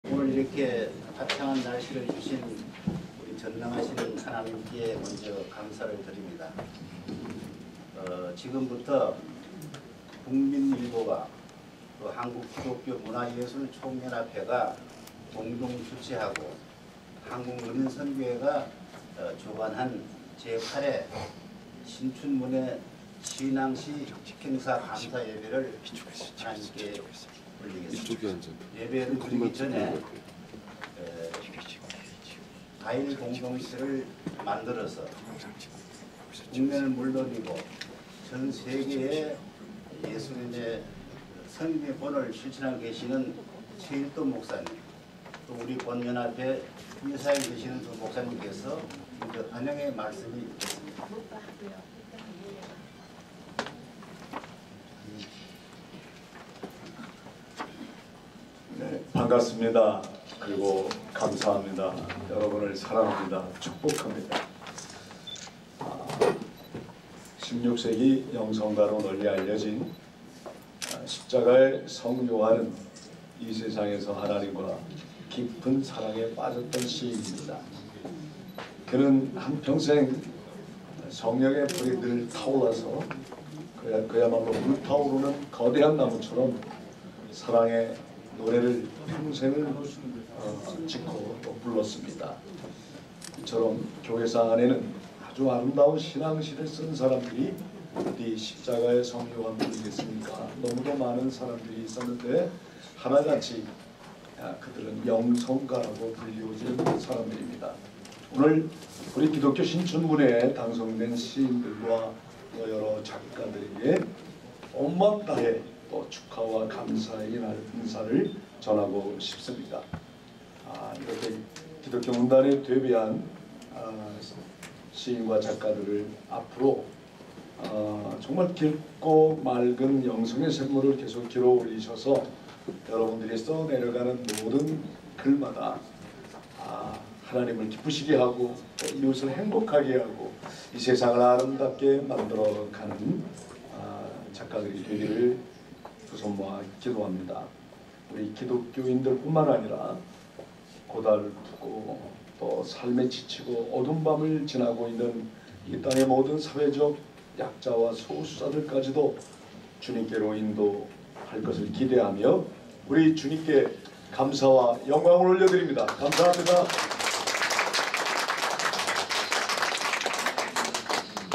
환영사...최일도(본회 이사장,다일공동체 대표)/제8회 국민일보 신춘문예 신앙시 감사예배 및 시상식
제8회 국민일보 신춘문예 신앙시 감사예배 및 시상식
1부 감사예배
        사회 : 김사철 장로(한국교회평신도지도자협회 초대회장)
        반주 : 이숙경 권사(찬양사역자)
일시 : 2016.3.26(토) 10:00
장소: 월드비젼 9층 강당(예배실)
주최 : 국민일보, 한국기독교문화예술총연합회
주관 : 한국문인선교회
후원 : CTS기독교TV, CBS기독교방송, CBS TV, FEBC 극동방송,
           하늘중앙교회, 신촌성결교회, 다일공동체